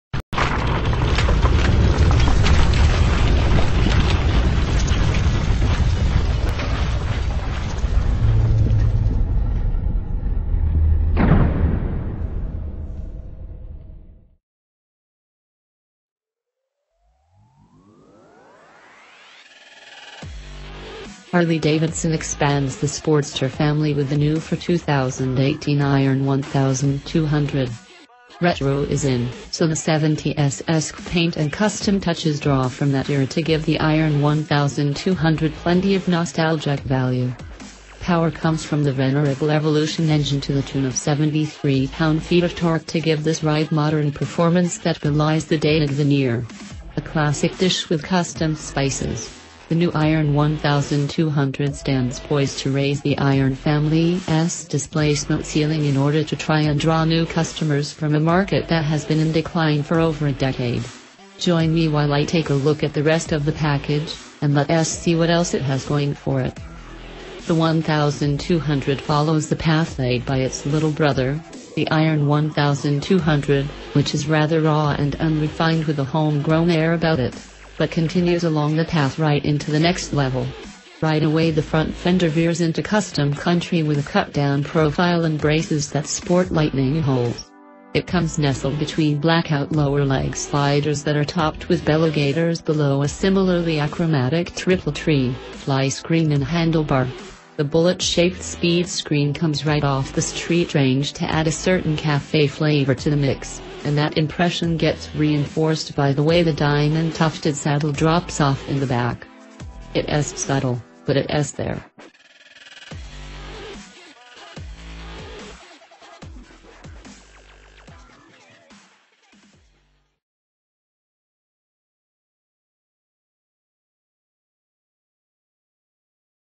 Otto Bike l 2019 Harley Davidson Iron 1200 Riding and Price Review
Otto Bike l 2019 Harley Davidson Iron 1200 Riding and Price Review-Harley-Davidson expands the Sportster family with the new-for-2018 Iron 1200. Retro is in, so the ’70s-esque paint and custom touches draw from that era to give the Iron 1200 plenty of nostalgic value. Power comes from the venerable Evolution engine to the tune of 73 pound-feet of torque to give this ride modern performance that belies the dated veneer. A classic dish with custom spices, the new Iron 1200 stands poised to raise the Iron family’s displacement ceiling in order to try and draw new customers from a market that has been in decline for over a decade. Join me while I take a look at the rest of the package, and let’s see what else it has going for it.

██████ GET DAILY UPDATES! NOW! ██████